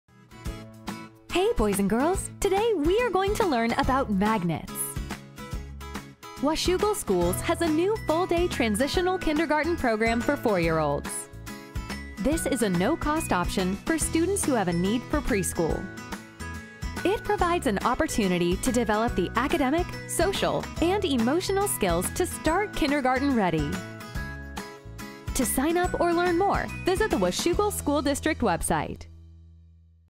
Transitional Kindergarten English
Transitional Kindergarten, also known as TK, is a no-cost early learning program, primarily for four-year-olds that is designed to help close the opportunity gap for students entering Kindergarten.  TK is available for students who have a demonstrated need, or when the family cannot afford access to a community program.  The TK program provides a high quality learning environment geared to help students build the skills they need to have a great start to Kindergarten.  Students must be four years old by August 31st to participate.